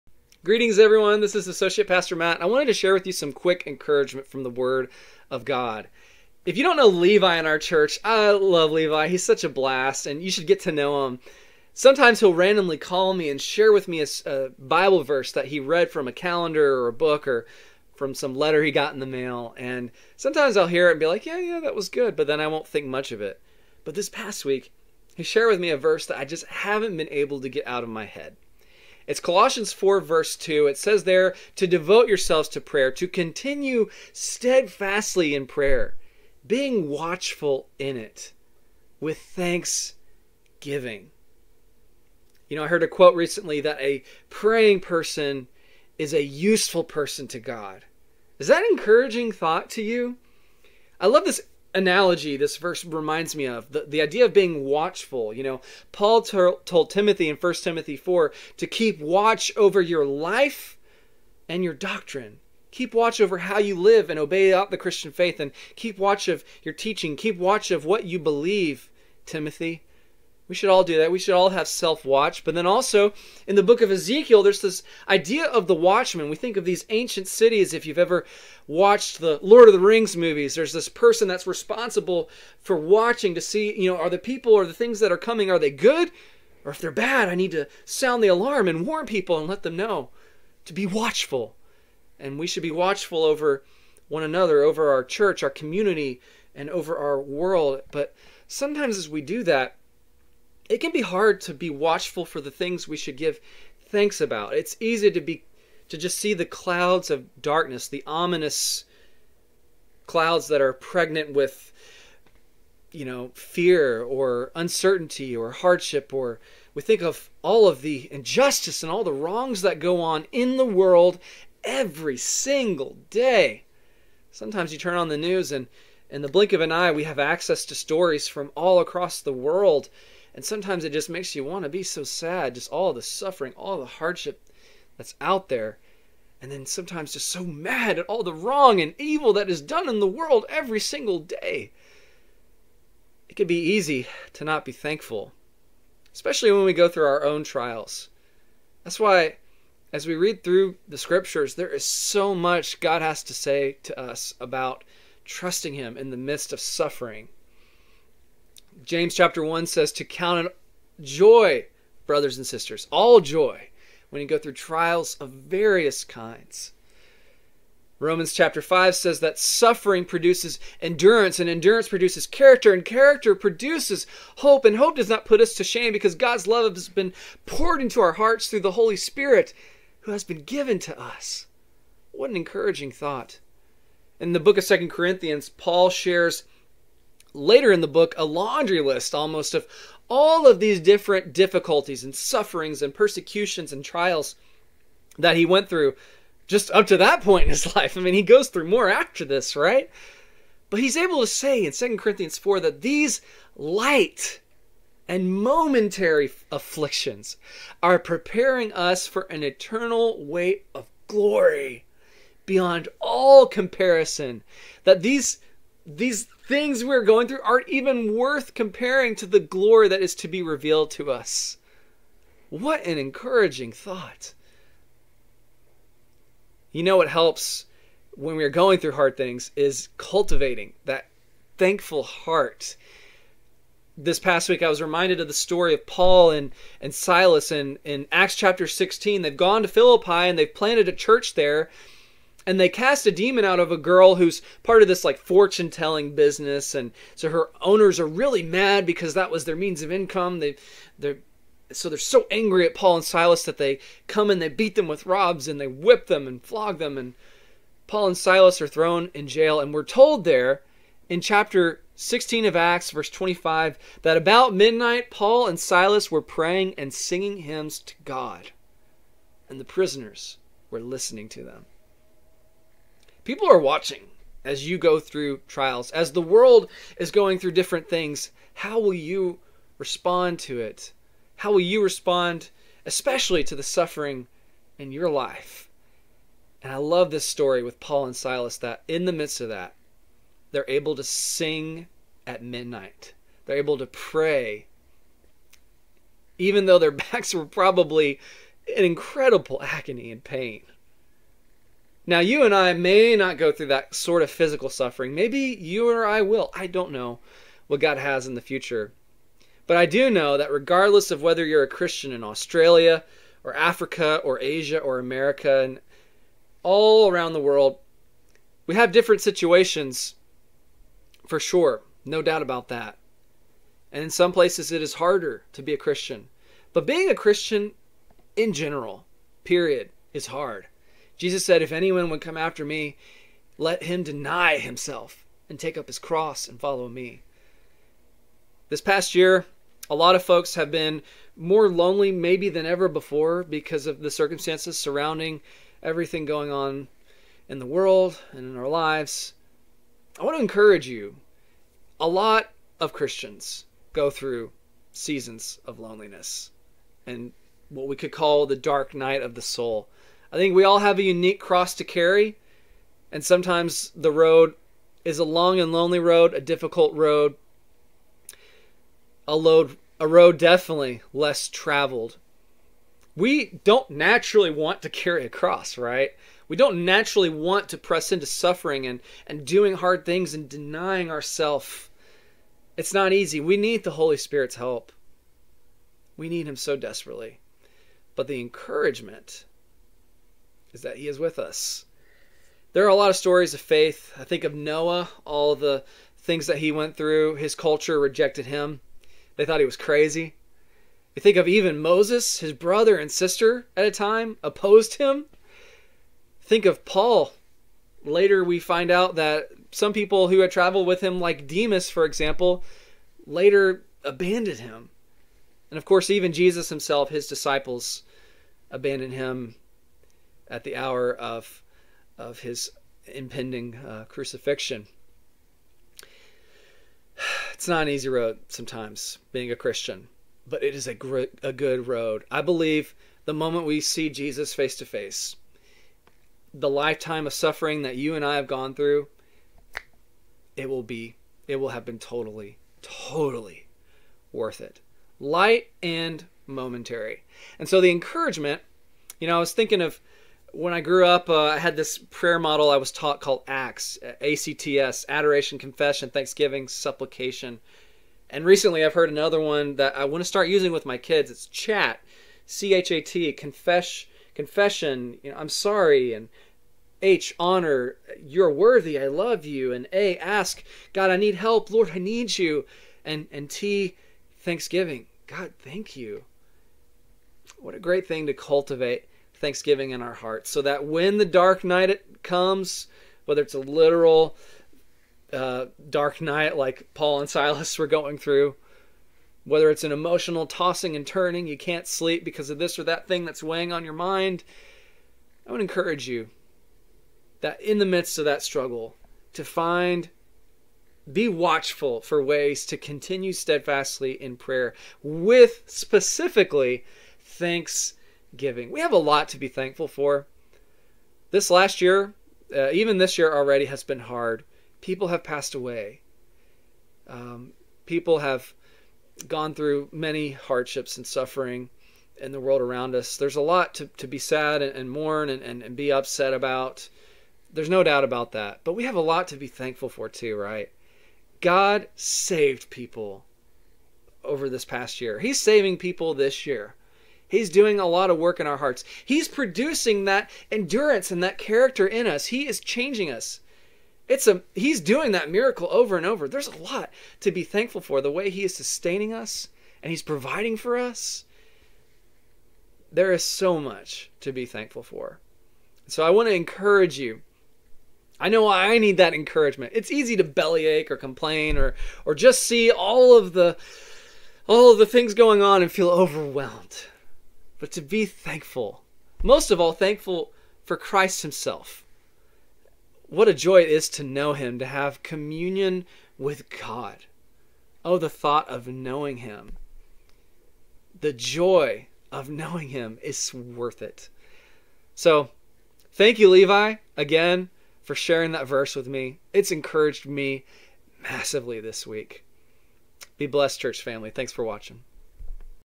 Devoted & Watchful
Quick encouragement from Colossians 4:2. God doesn't waste a millisecond of our suffering, and  God uses praying people!  Keep steadfast and trust in Him!